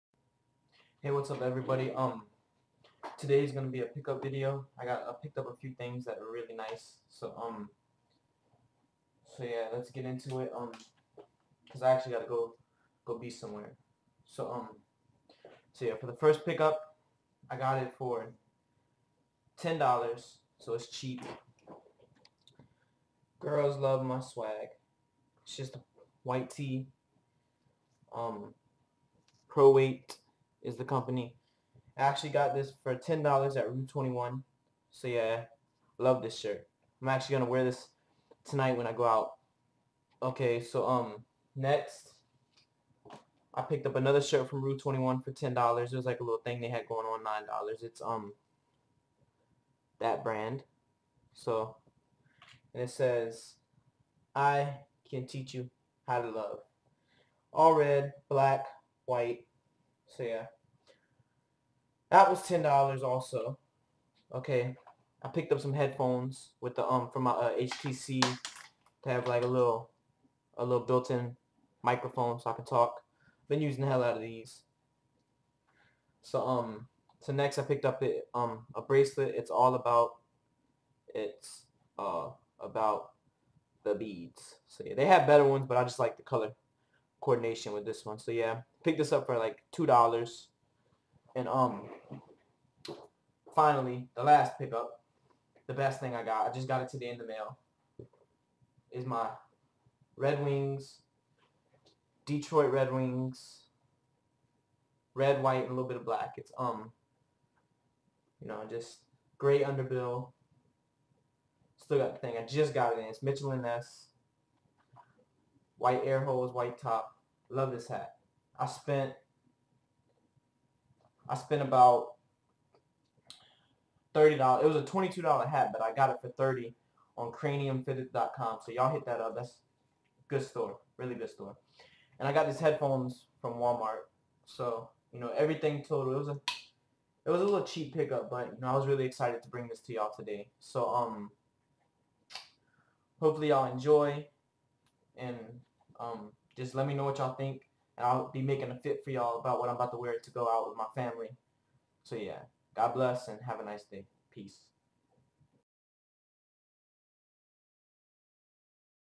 Pickups #2 Snap Back + Shirts + Bracelet
This is a quick pick up did for you guys.Hope you enjoy ;)

Red Shirt: Rue 21 for $10
White Shirt: Rue 21 for $10
Bracelet: Rue 21 for $2
Snap Back: Cranium fitteds for 30

GOD BLESS ;))